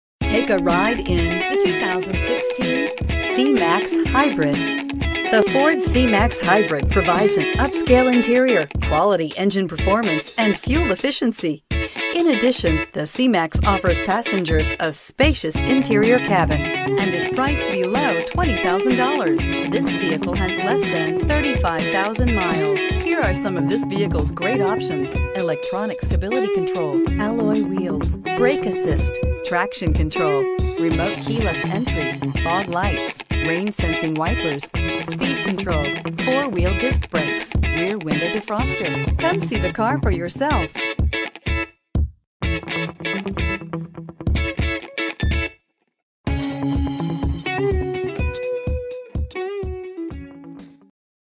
2016 Ford C-Max Hybrid SEL FOR SALE in Flagstaff, AZ 74215
Stop in today for a test drive of this 2016 Ford C-Max Hybrid

Tectonic 2016 Ford C-Max Hybrid SEL FWD CVT I4 Hybrid C-Max Hybrid SEL, 4D Hatchback, I4 Hybrid, CVT, FWD.  Odometer is 3068 miles below market average! 42/37 City/Highway MPG  Awards:     2016 KBB.com Brand Image Awards